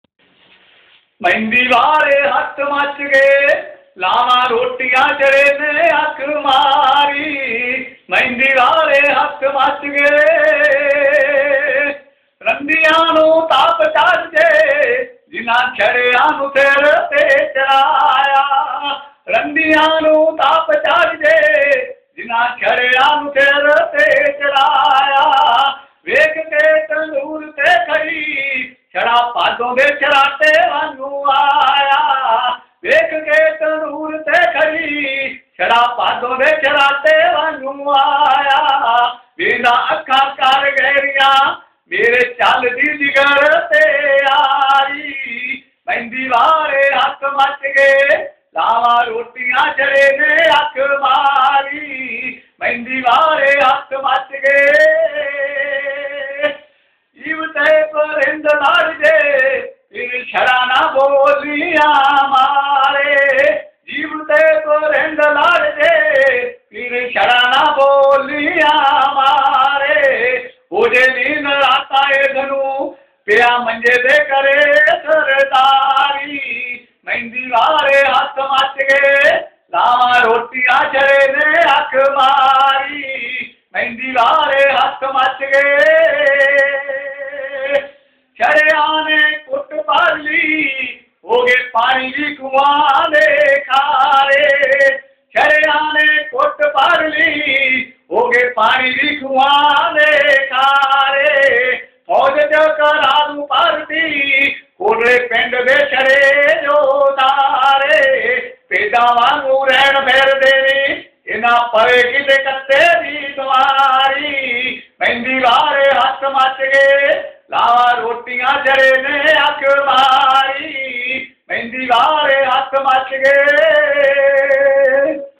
punjabi's in sudan.3gp
22 g from kotla performing on thursday night in Abu hamama in Khartoum, Capital of sudan wid frnds, chancal, lakha, janta, balvir,sunny,sidhu...
written nd sung by saday respected 22 g from  village Kotla , punjab..